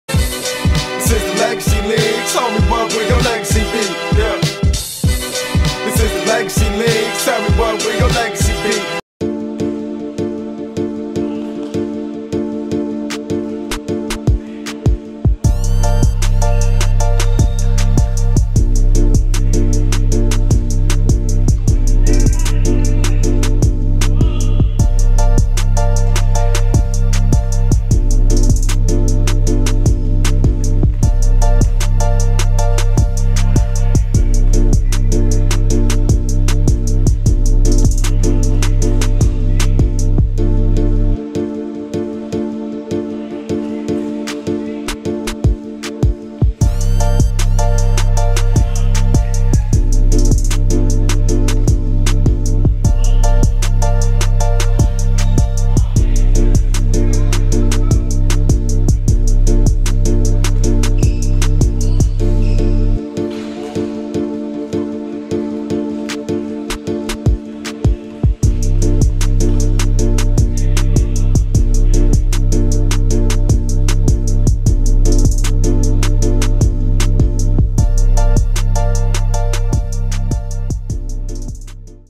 Shake N' Bake Swish Kabobs RI Highlights - Summer 18
Shake N' Bake vs Swish Kabobs highlights from Week 7 of the Rhode Island Legacy Leagues Summer 18 season. Swish Kabobs won 63-58.